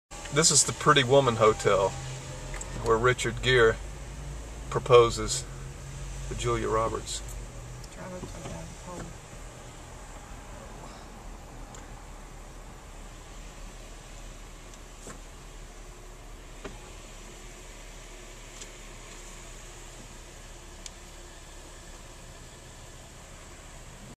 "Pretty Woman" - Hotel Fire Escape
The Las Palmas Hotel is where the final scene of the movie "Pretty Woman" with Julia Roberts and Richard Gere was filmed.   This was videod on April 24, 2013.